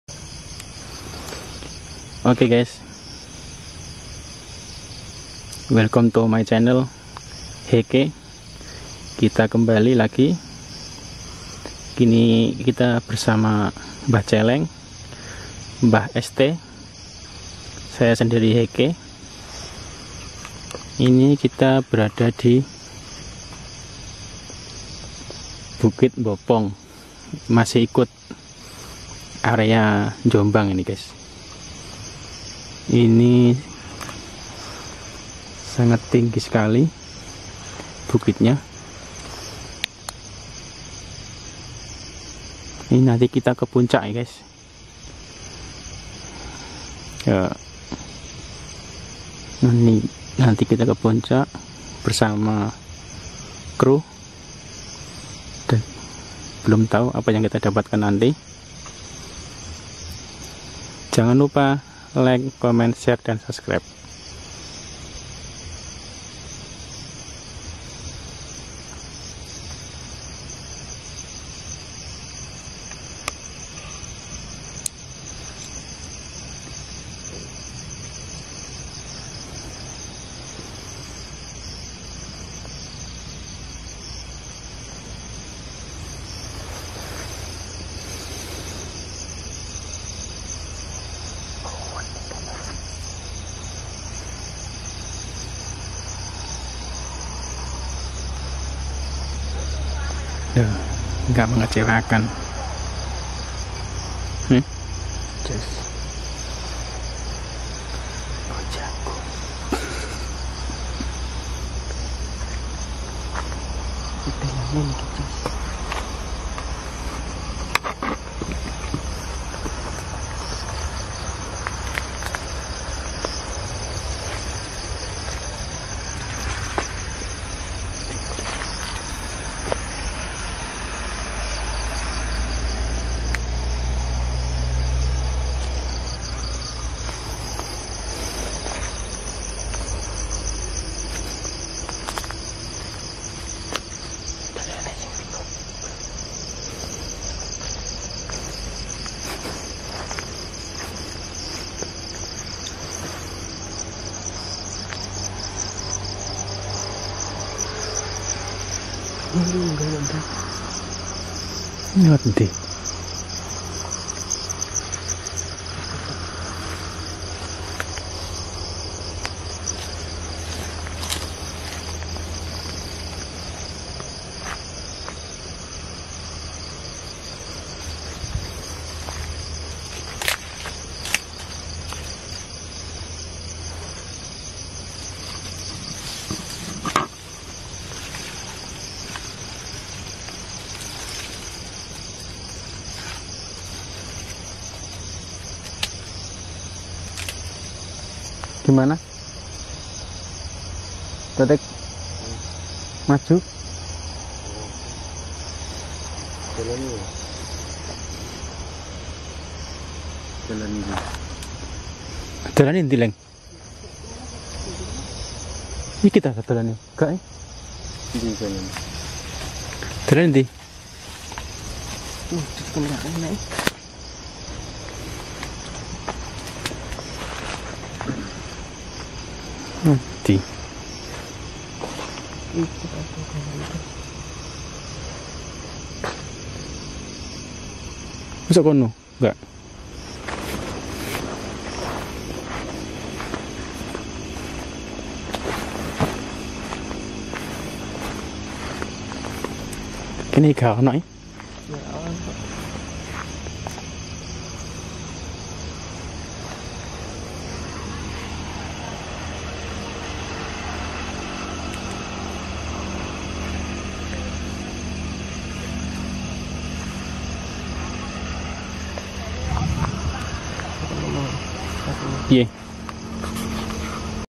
Makam keramat di bukit bopong jombang jatim
DOMNO KONCO

Di Ds. Bopong Kec. Kabuh Kab. Jombang jawatimur

Terdapat banyak sekali perbukitan, salah satu adalah bukit yang di beri nama Bukit Bopong!

Bukit tersebut terdapat makam yang masih di keramatkan oleh beberapa orang di daerah sekitar maupun luar daerah tersebut..

Ini awal perjalanan kami untuk sampai ke atas bukit tersebut..
Mohon doa nya ya dulur. semoga lancar lancar aja,
Aamiin.

Kalau ada yg kurang dan lebih nya mohon tulis di kolom komentar aja ya dulur. makasih